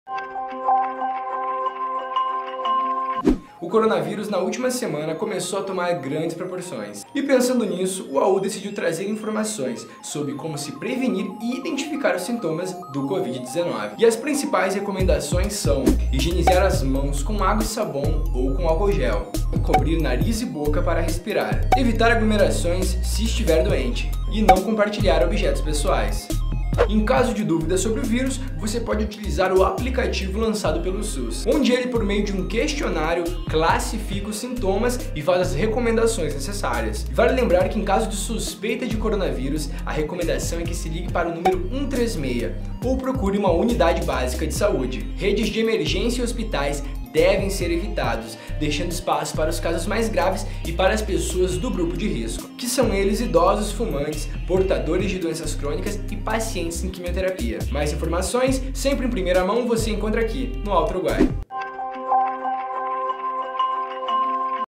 CORONAVÍRUS | SINTOMAS E PREVENÇÃO DO COVID-19
O jornal O Alto Uruguai, apresenta de um modo rápido quais as principais precauções e os sintomas do novo CORONAVÍRUS. informação sobre o 136 e o novo aplicativo do SUS que monitora o COVID-19. CORONAVÍRUS COVID-19